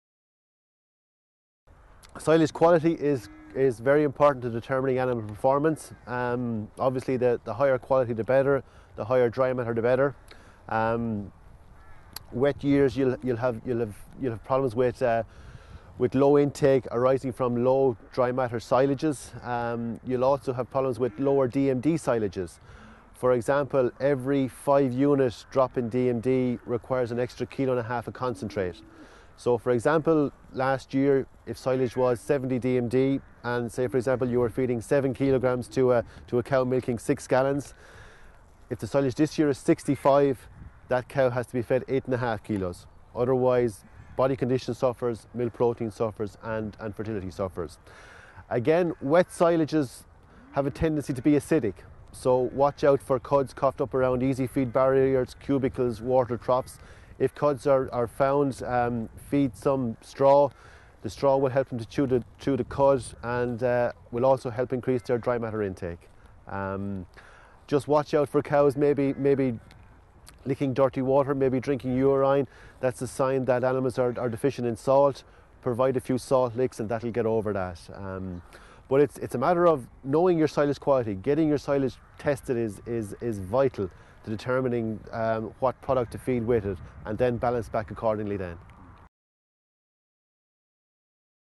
GAIN Ruminant Feeds - Silage Quality
Silage quality is very important as it effects animals performances. The higher quality the better. Wet years you will have problems with low intake, DMD silage. Wet silage can be acidic. Straw will help with their dry matter intake. Watch out for cows licking dirty water or drinking urine, its a sign that they are defficiant in salt. Provide salt licks to prevent this.